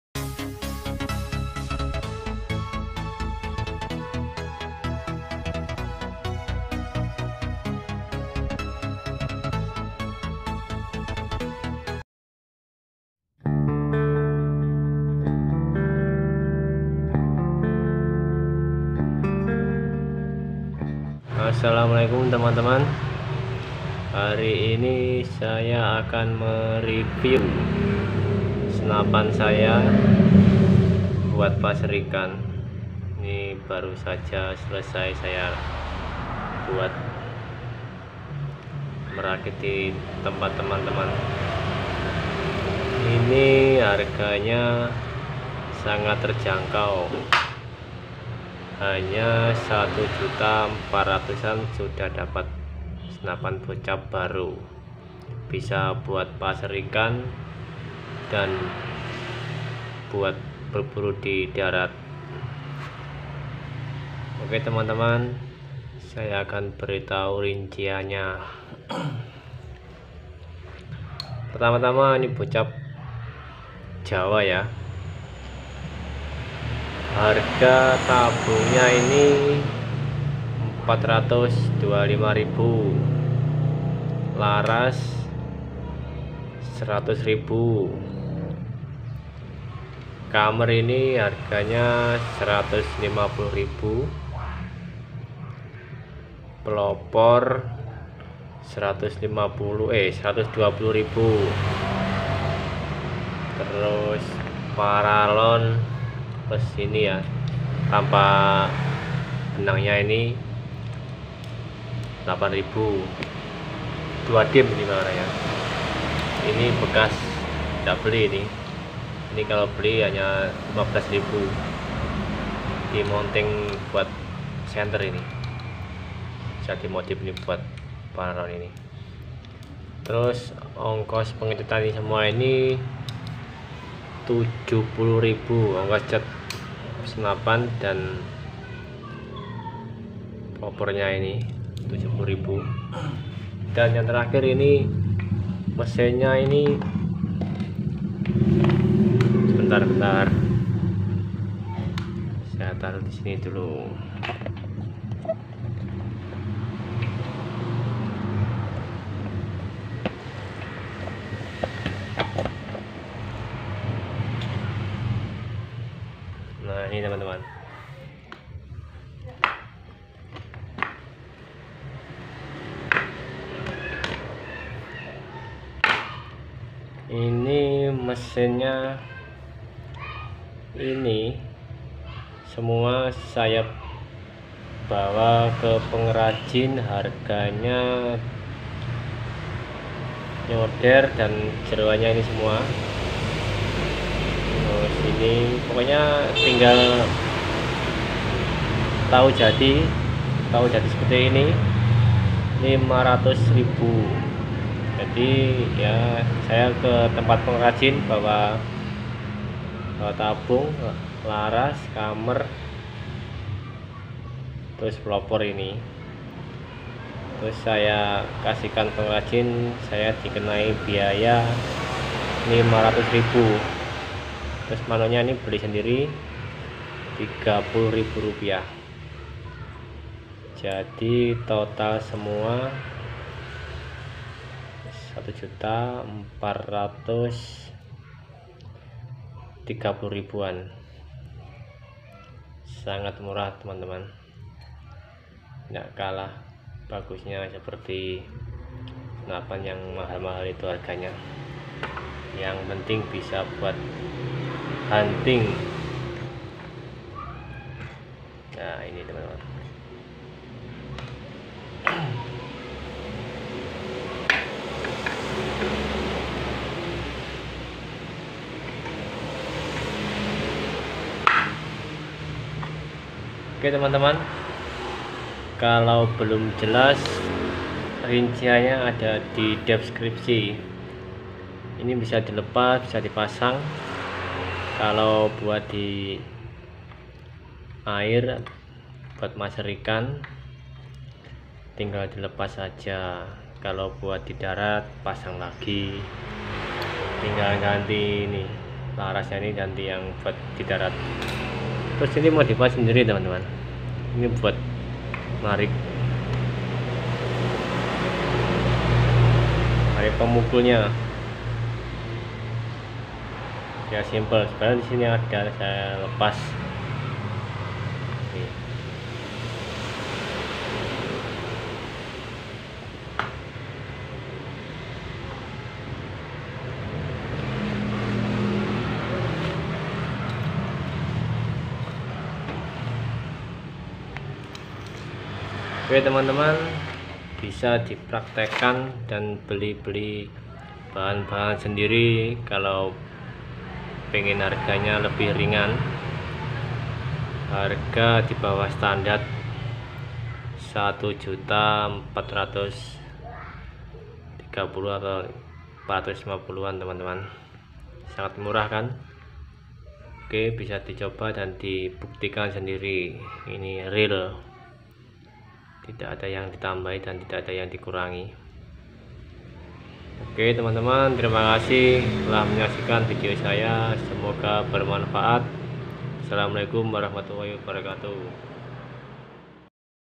Paser ikan murah tapi tidak murahan
Tabung bocap jenong =425k
Laras = 100k
Camer = 150k
Plopor = 120k
paralon = 8k
biaya cat = 70k
Mano= 30k
Biaya ngerakit di pengerajin = 500k